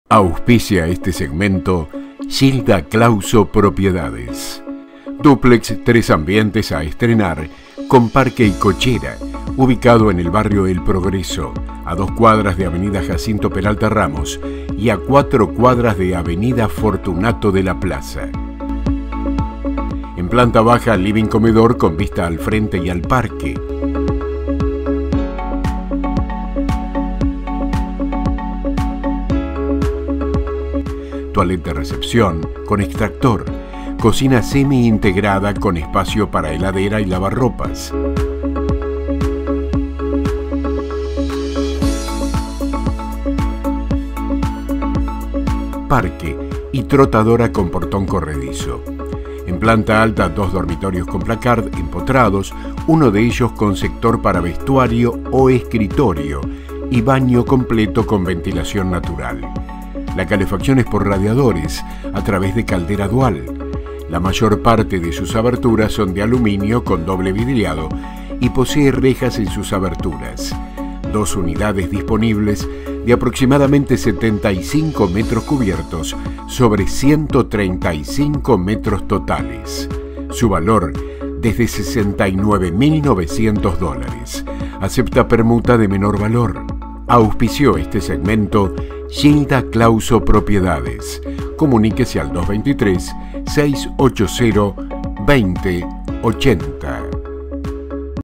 Gilda Clauso - El Progresa - Duplex 3 amb - A Estrenar -
DUPLEX 3 AMBIENTES A ESTRENAR CON PARQUE Y COCHERA, UBICADO EN EL BARRIO EL PROGRESO, A 2 CUADRAS DE AV JACINTO PERALTA RAMOS Y A 4 CUADRAS DE AV FORTUNATO DE LA PLAZA.
EN PLANTA BAJA: LIVING COMEDOR CON VISTA AL FRENTE Y AL PARQUE, TOILETTE DE RECEPCION CON EXTRACTOR, COCINA SEMI INTEGRADA CON ESPACIO PARA HELADERA Y LAVARROPAS, PARQUE Y TROTADORA CON PORTON CORREDIZO
EN PLANTA ALTA: 2 DORMITORIOS CON PLACARD EMPOTRADO UNO DE ELLOS CON SECTOR PARA VESTUARIO O ESCRITORIO Y BAÑO COMPLETO CON VENTILACION NATURAL
LA CALEFACCION ES POR RADIADORES A TRAVES DE CALDERA DUAL, LA MAYOR PARTE DE SUS ABERTURAS SON DE ALUMINIO CON DOBLE VIDRIADO Y  POSEE REJAS EN SUS ABERTURAS.
2 UNIDADES DISPONIBLES DE APROX 75 MTS CUBIERTOS SOBRE 135 MTS TOTALES.
SU VALOR DESDE US$ 69.900 - ACEPTA PERMUTA DE MENOR VALOR